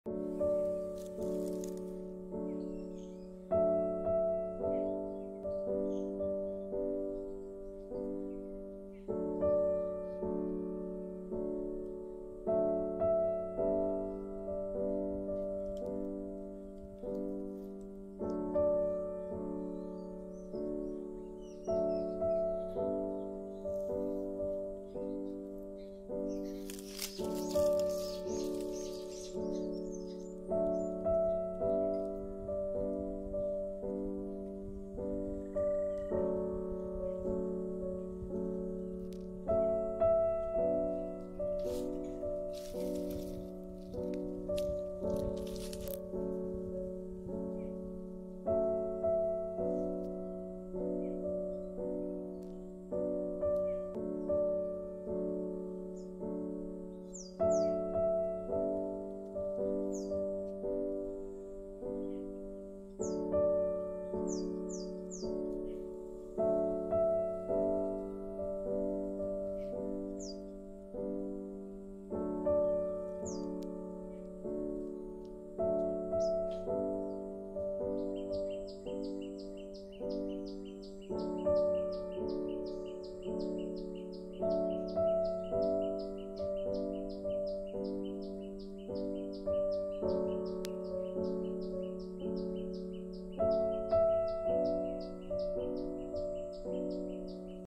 Honeybees are collecting honey from wild flower.
Honey is collected by the worker bee... for making royal jelly...